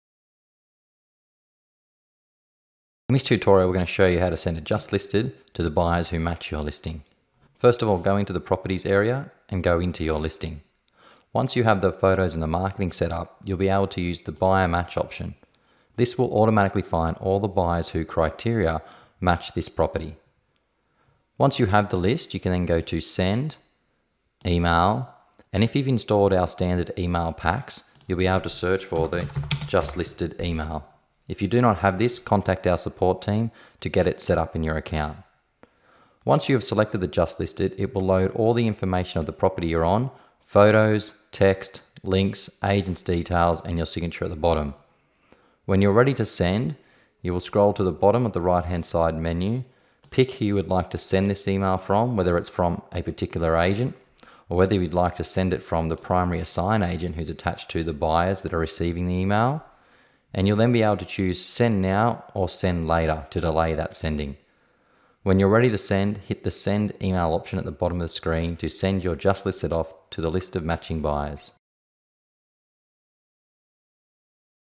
Sending Just Listed Email to Matching Buyers
In this tutorial, You will learn how to send a just listed email to all the buyers who match this property.